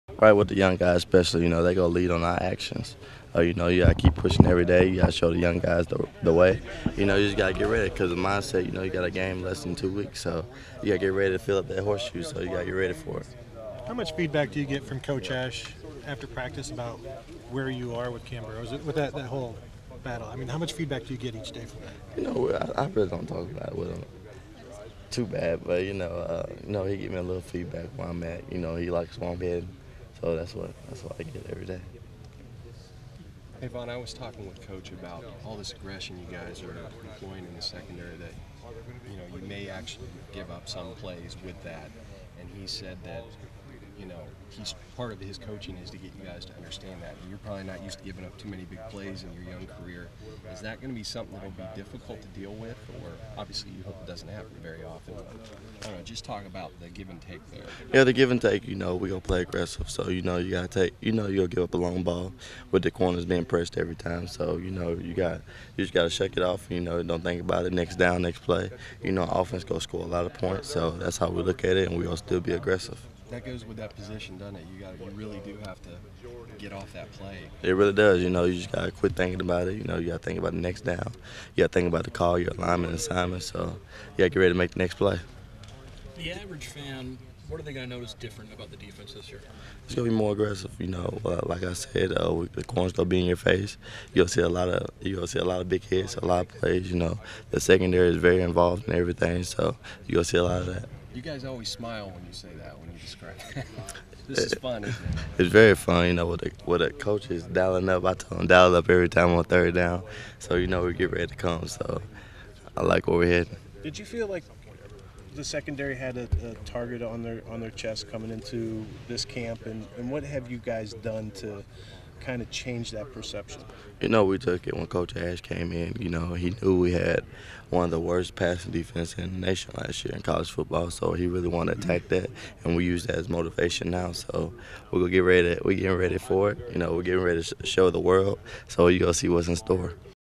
Ohio State So. DB Vonn Bell 8/18/2014
Buckeyes So. Safety on being a leader, the new aggressive D and more